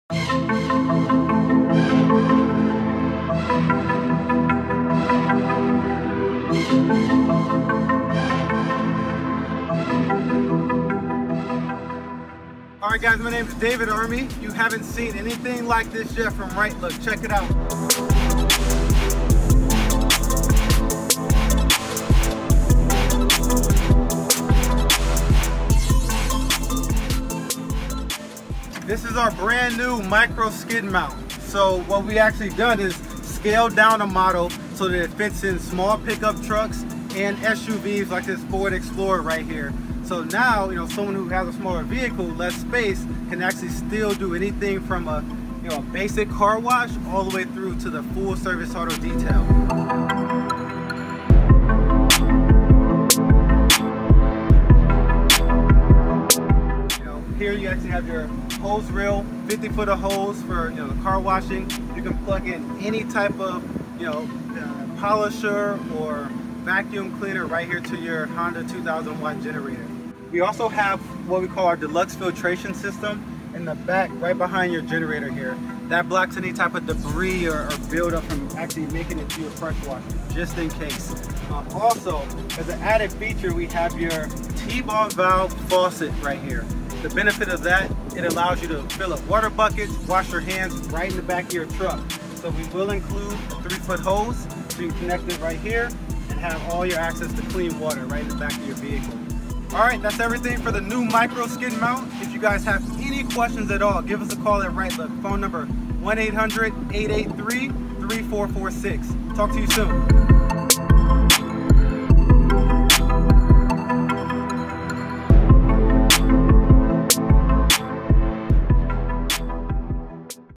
Micro Auto Detailing Skid Mount Unit
Check out this recent video where David shows off our brand new Micro Skid Mount Unit. This skid mount is small, but mighty. It comes outfitted with a 60 gallon water tank, 50 foot hose, 2000 watt quiet generator, and a 2000 PSI pressure washer. This unit is small enough to fit in the back of a small truck or SUV, but powerful enough for everyday use by the professional mobile auto detailer.